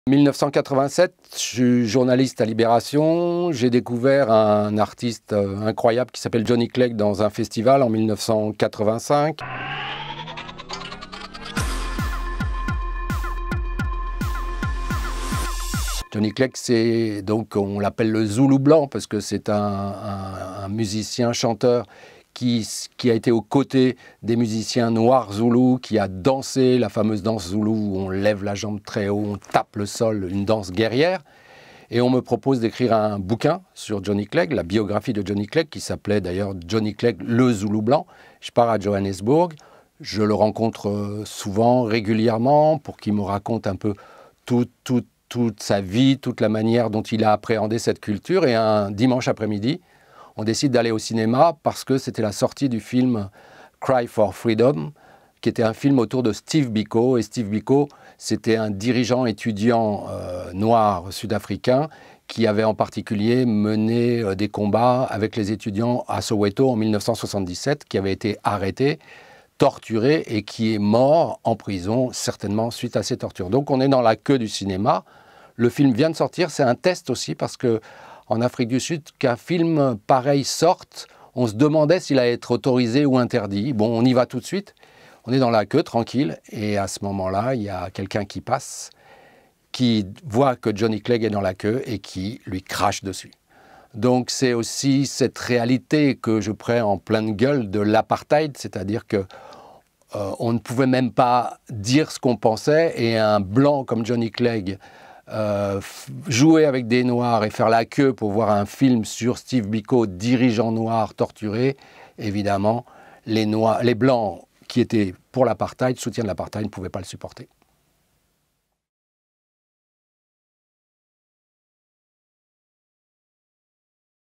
KbdK On Air / POP / Philippe Conrath épisode 1 : le Zoulou blanc
Les multiples vies de Philippe Conrath (journaliste, écrivain, patron de label, directeur de festival, manageur d'artistes) en 8 épisodes.
1er épisode : dans une queue de cinéma en Afrique du Sud avec Johnny Clegg.